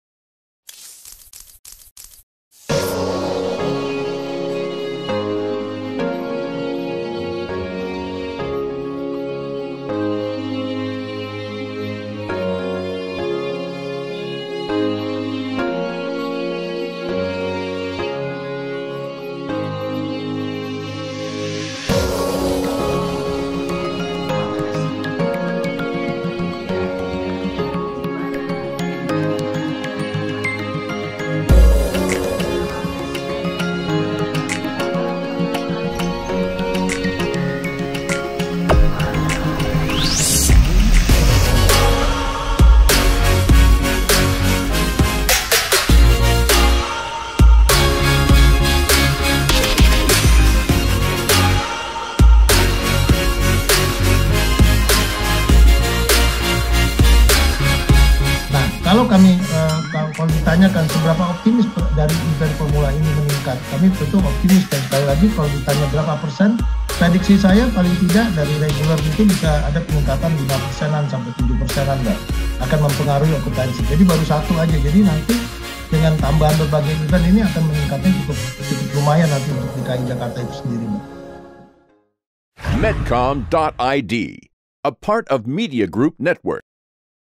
Formula E Diyakini Dapat Mempercepat Pemulihan Sektor Pariwisata
Sekjen Perhimpunan Hotel dan Restoran Indonesia, Maulana Yusran mengatakan penyelenggaraan acara internasional seperti balap mobil Formula E dapat mempercepat pemulihan sektor pariwisata yang saat ini mulai bangkit.

Menurut Maulana, kedatangan peserta balap hingga penonton akan mendorong tingkat okupansi hotel maupun aktivitas belanja pada saat sebelum maupun sesudah pelaksanaan Formula E. Pelaku pariwisata di Jakarta juga sudah menyediakan paket wisata terkait Formula E. Sehingga, Maulana memprediksi pelaksanaan kegiatan Formula E bakal mendorong tingkat okupansi hotel naik 5 hingga 7 persen.